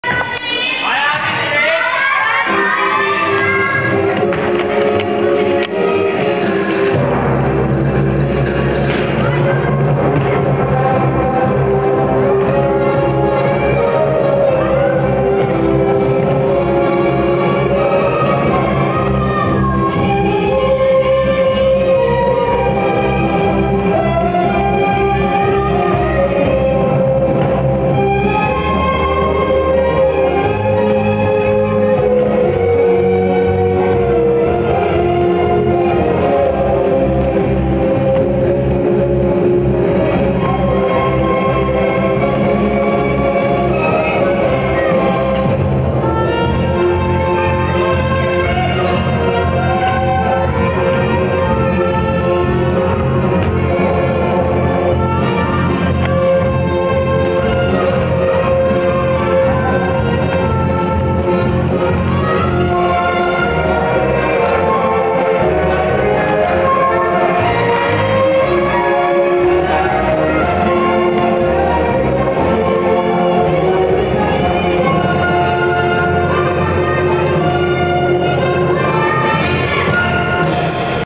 László Csaba - Mikes Anna(10tánc MB, paso doble)
Felnőtt Tíztánc Magyar Bajnokság, Vác, 2009. október 3.
László Csaba és Mikes Anna pasója:) (L)
Hungarian Championships Tendance Adult, Paso Doble with Csaba László and Anna Mikes
3 october 2009, Vác